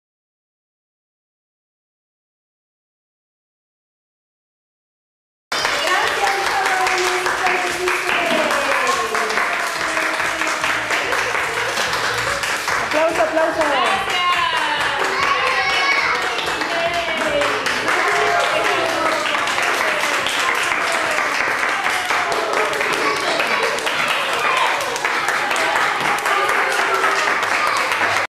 Aplauso para el Papa Benedicto XVI del Centro Escolar Las Charcas (Guatemala)
Descripción: El aplauso más largo es una sencilla iniciativa: realizar un vídeo con aplausos de personas de los cinco continentes para enviárselo al Papa el 28 de febrero a través de su cuenta de twitter. y en la cuenta de twitter @aplausoBXVI